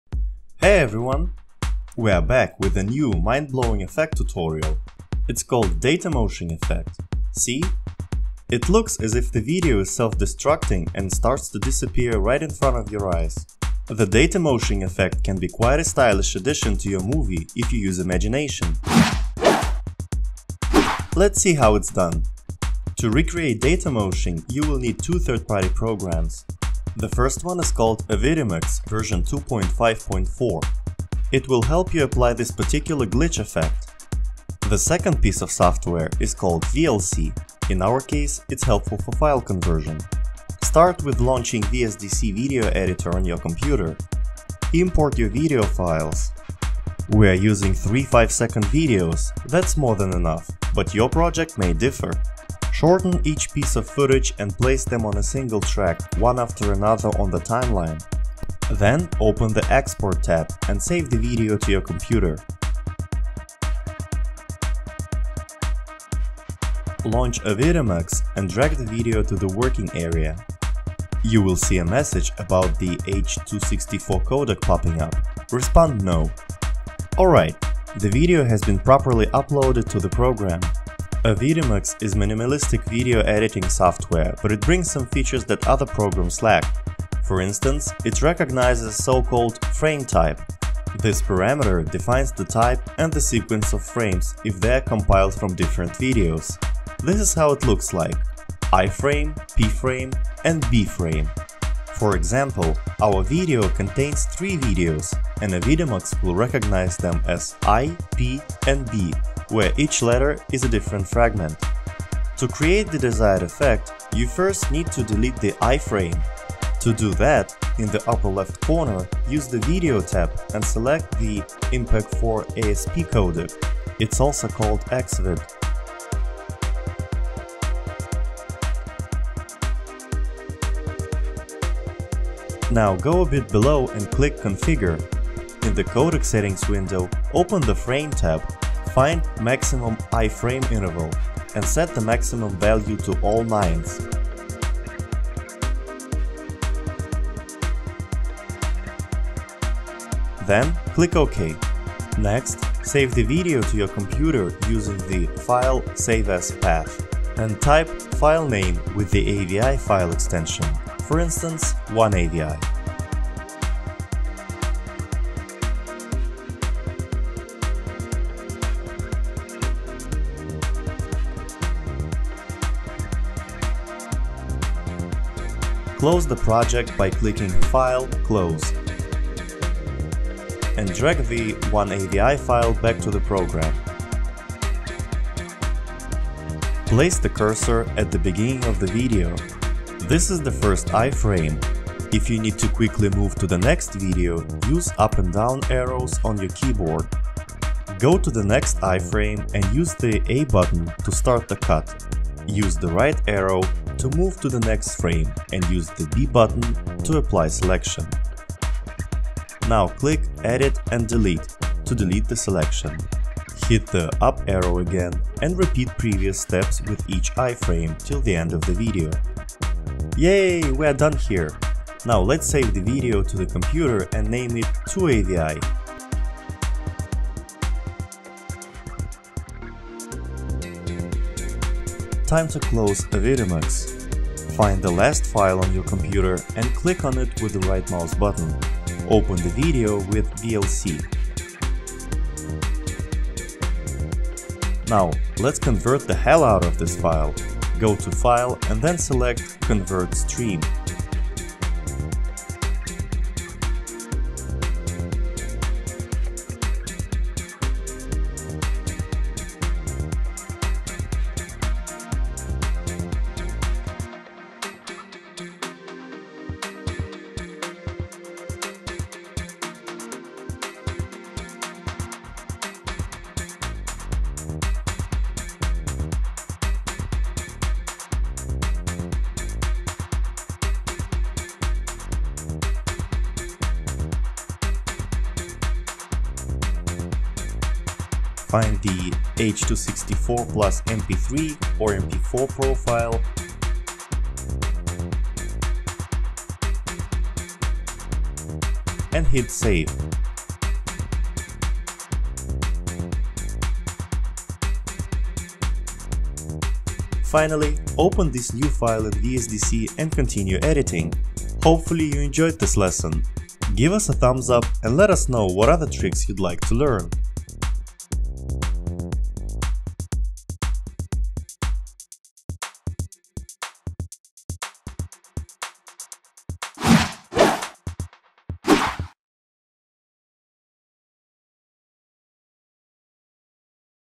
How to create Datamoshing effect for free: tutorial
The datamoshing effect looks as if the video is self-destructing and starts to disappear right in front of your eyes. It can be quite a stylish addition to your movie if you use your imagination! In this tutorial, we show you how to recreate the datamoshing effect for free using VSDC, Avidemux, and VLC.

Before you start, make sure to download the latest versions of the above-mentioned programs. Avidemux will help you apply the datamoshing effect, and VLC will help to convert the file properly.